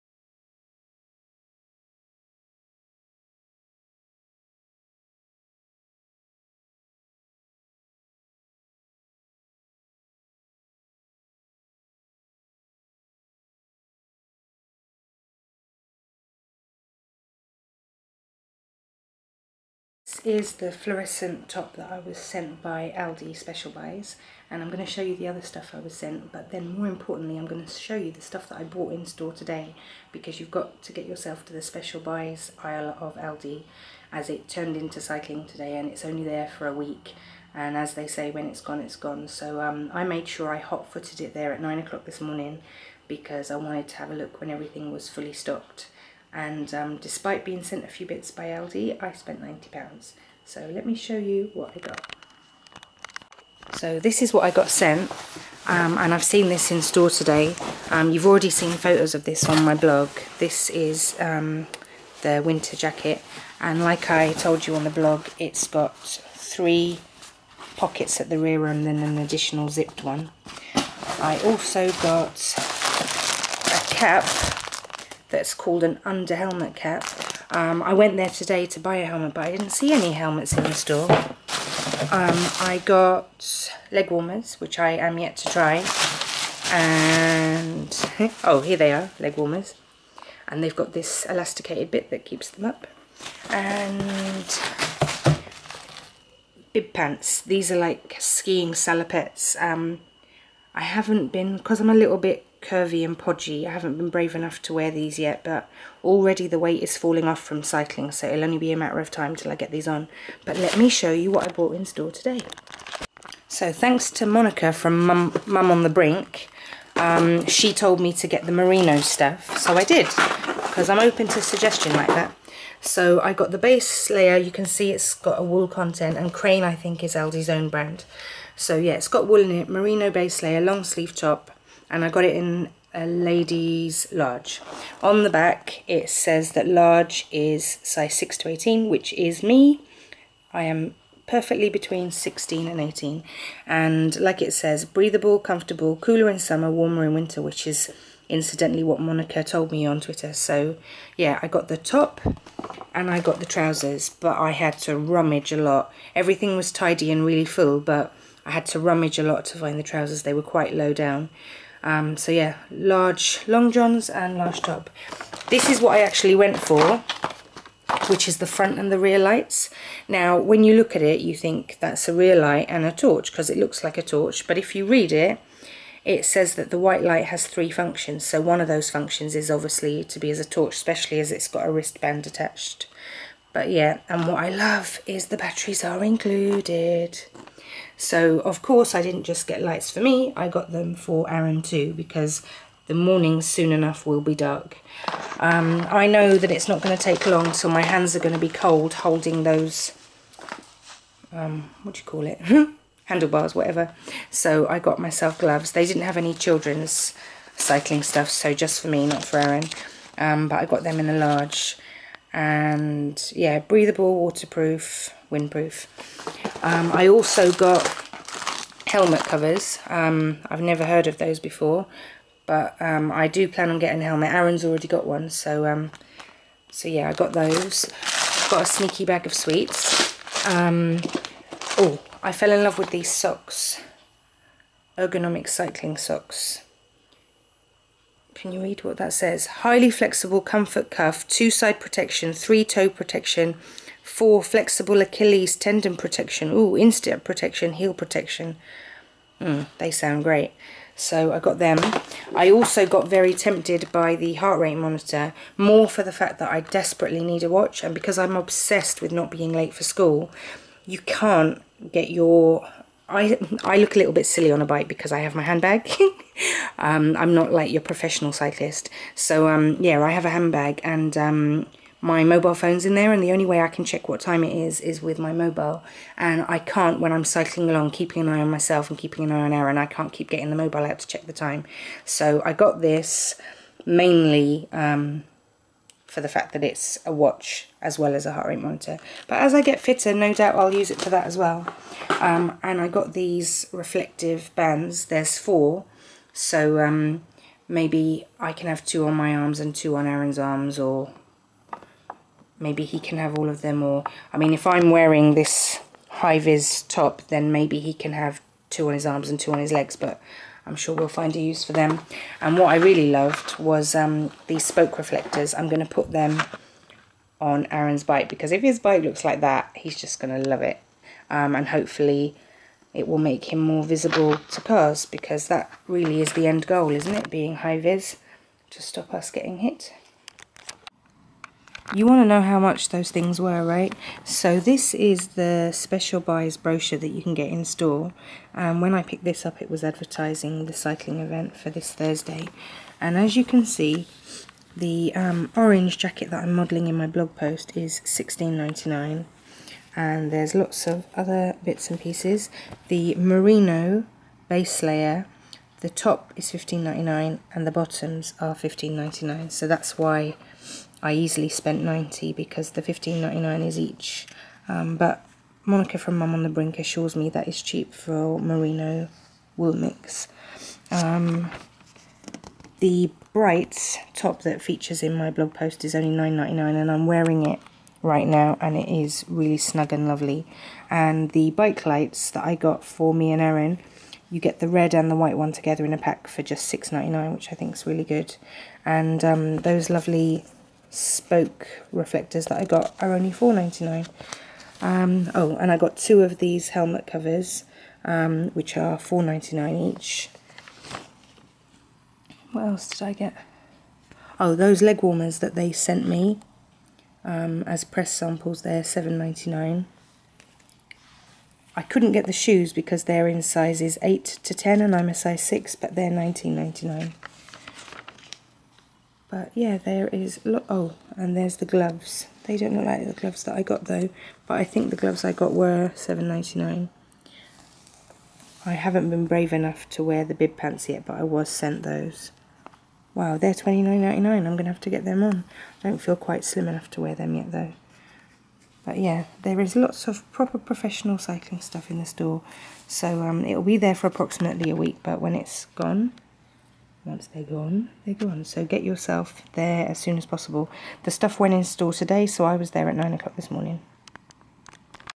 My Aldi Special Buys Cycling Aisle HAUL
A haul from the Special Buys aisle of Aldi UK. Me and my son are cycling to and from school every day, and plan to in all weathers even when the mornings get dark hence me hot footing it there today for bike lights. Get all things cycling related in +AldiUK now, for bargain prices! Full review is over on newmumonline.co.uk Thanks x